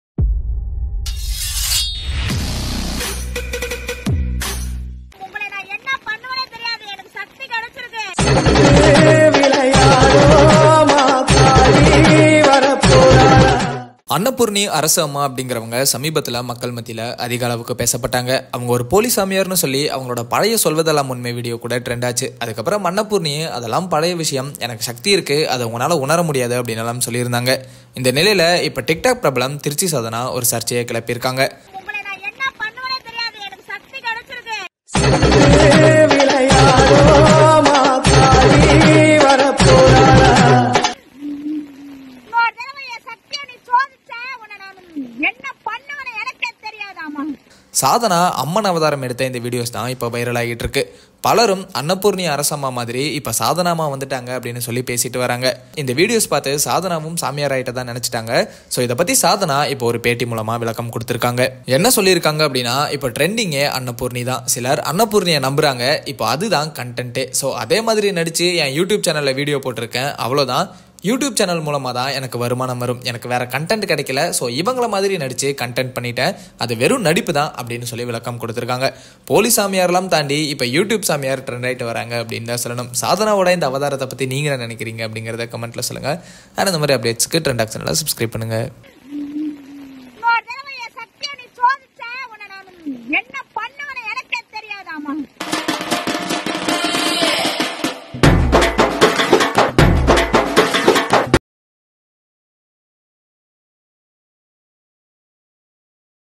Trichy Sathana-வின் திடீர் அம்மன் அவதாரம்.. அடுத்த Annapurani Arasu Amma? - Interview | Amman Videos
Trichy Sathana-வின் திடீர் அம்மன் அவதாரம்.. அடுத்த Annapurani Arasu Amma? - Interview | Amman Videos | Trichy sathana latest video | trichy sathana polimer news